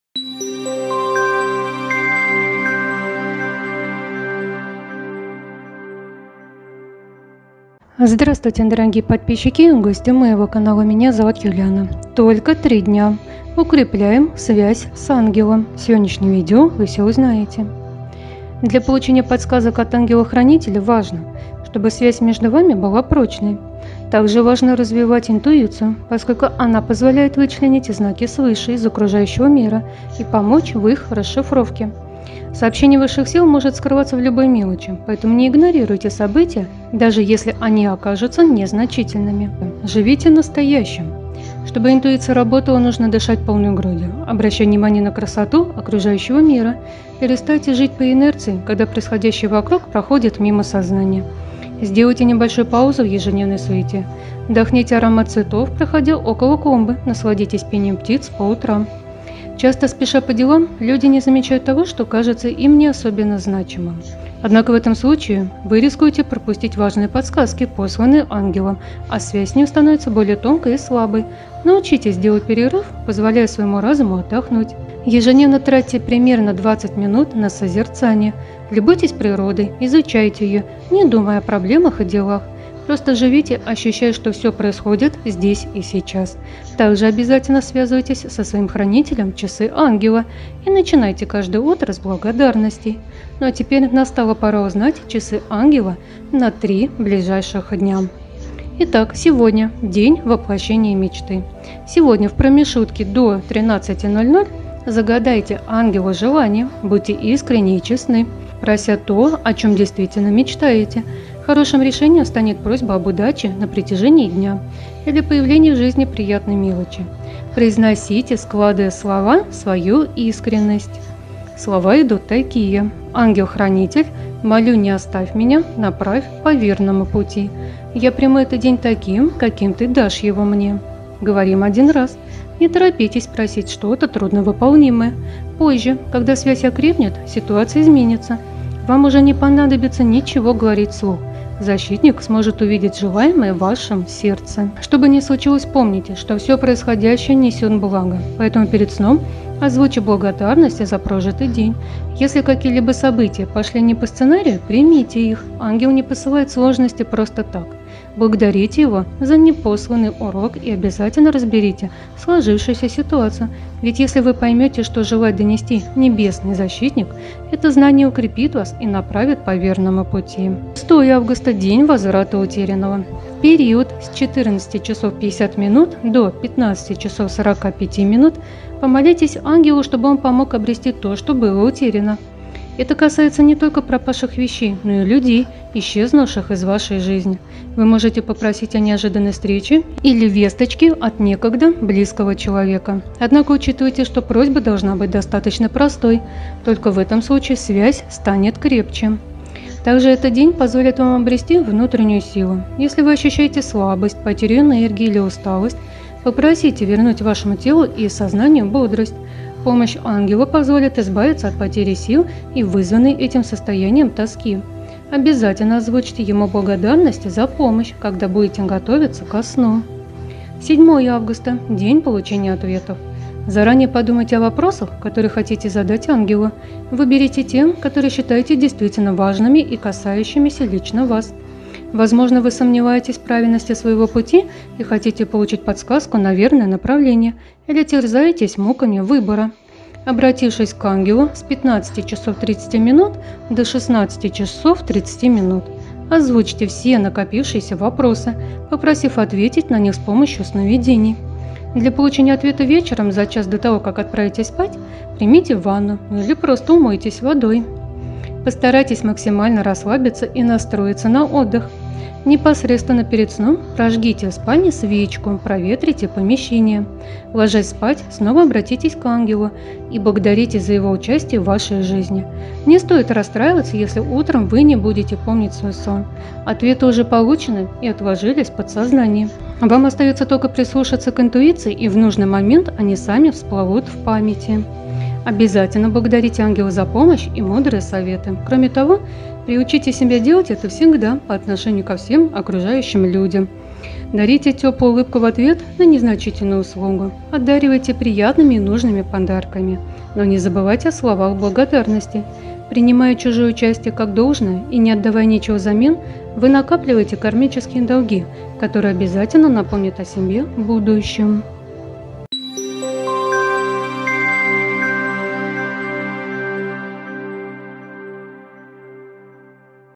Только 3 дня - укрепляем связь с Ангелом.
Сегодня хочу с вами поделиться и рассказать о предстоящих днях, когда  можно укрепить связь с Ангелом и попросить о важном.

Благодарю Вас!

💰 Вы можете оказать поддержку проекту, любым удобным способом.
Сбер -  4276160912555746